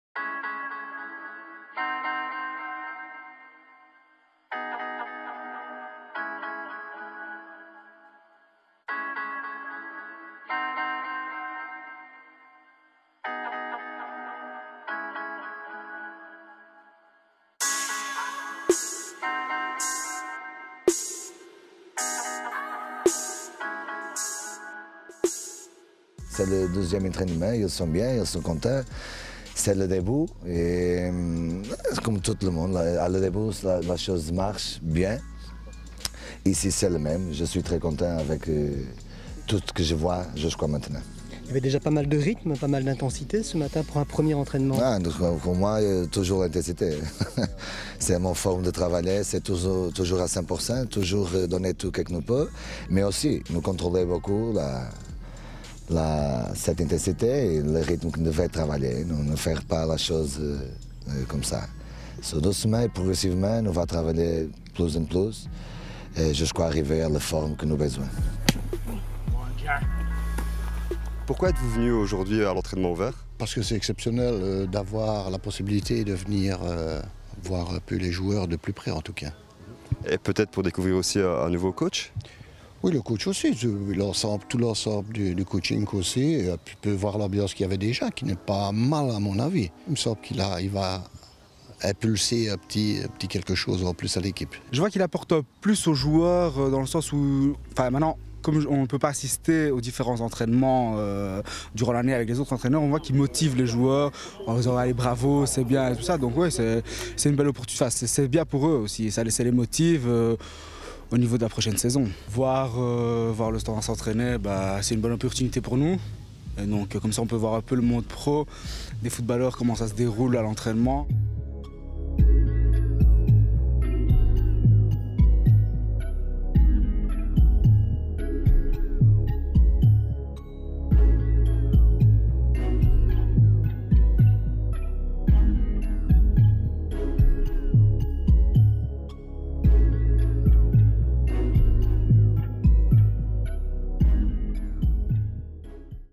Entraînement ouvert au Standard de Liège
Ce mercredi matin, Ricardo Sá Pinto et son staff ont donné un entraînement ouvert au public.